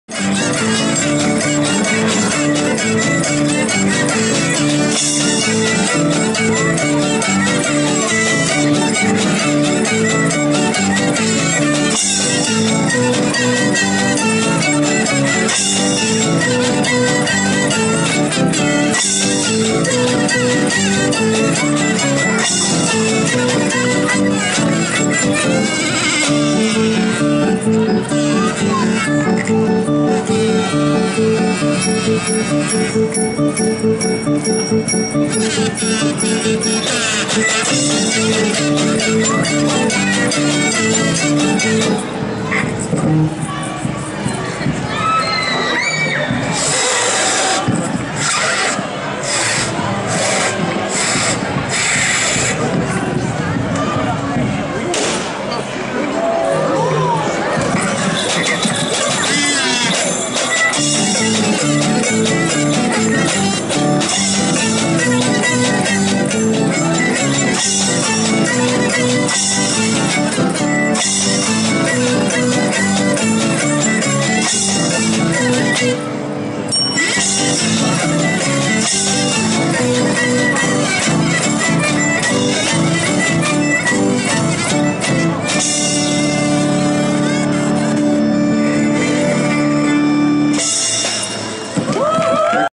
EPPAI Festival Fringe Edinburgh Scotland
Tour Scotland video of EPPAI performing outside St Giles Cathedral on the Royal Mile on visit to the Festival Fringe in Edinburgh